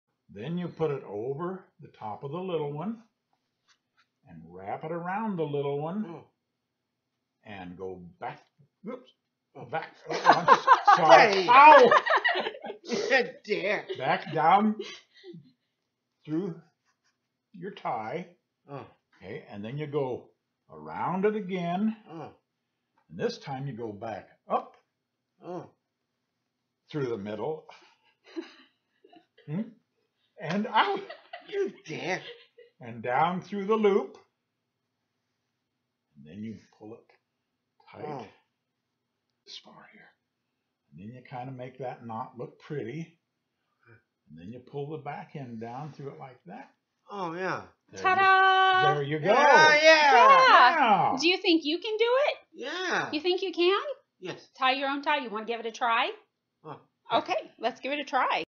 Dad teaches Berkeley how to tie a necktie
Dad teaches Berkeley, who has Down syndrome, how to tie his necktie.